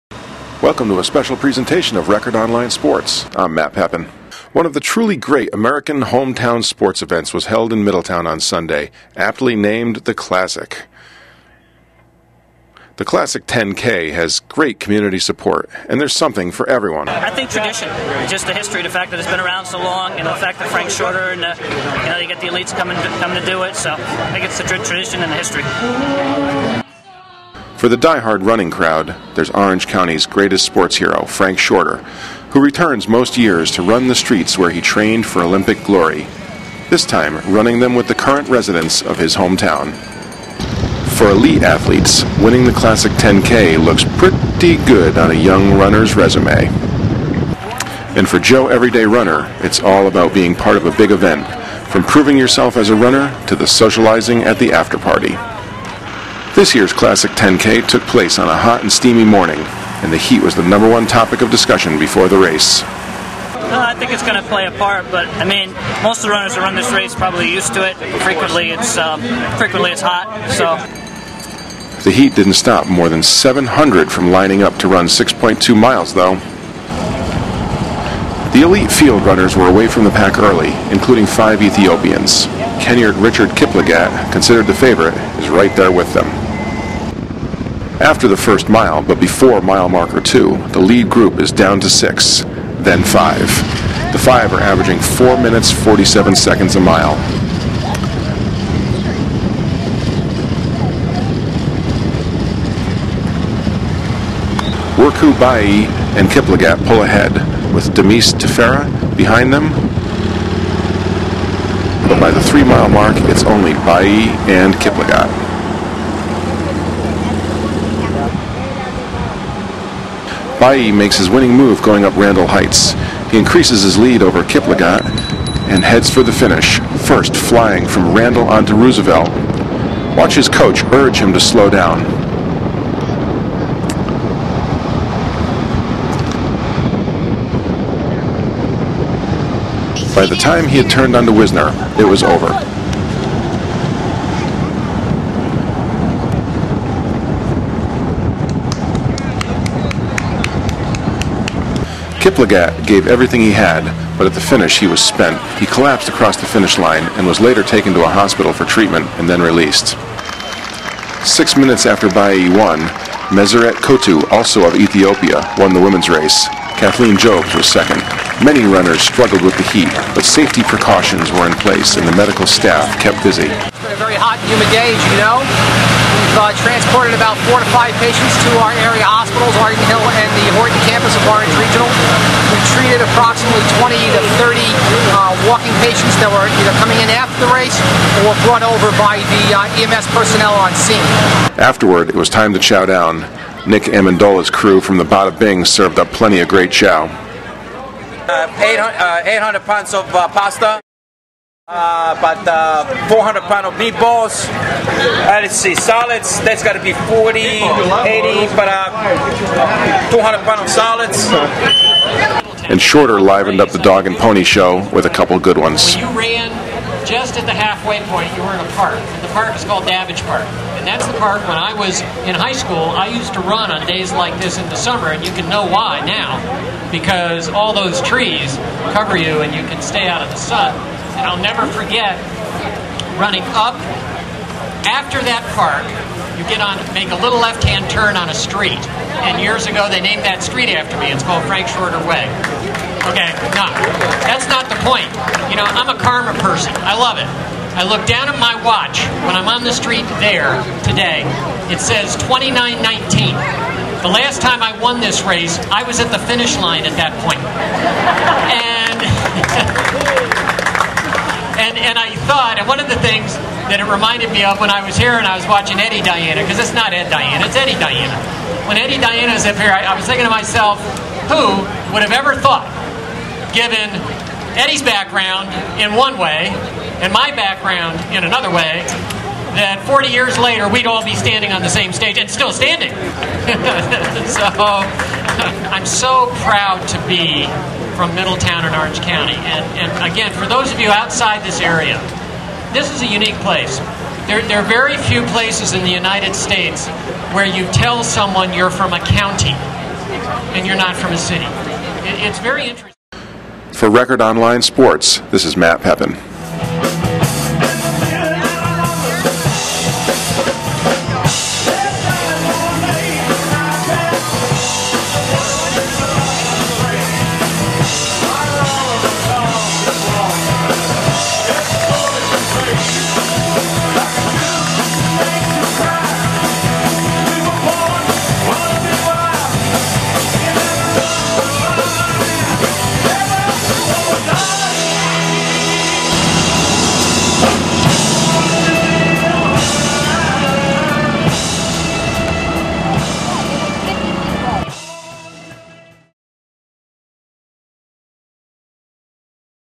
Classic 10K road race
Highlights from the 2008 Classic 10K road race in Middletown, N.Y.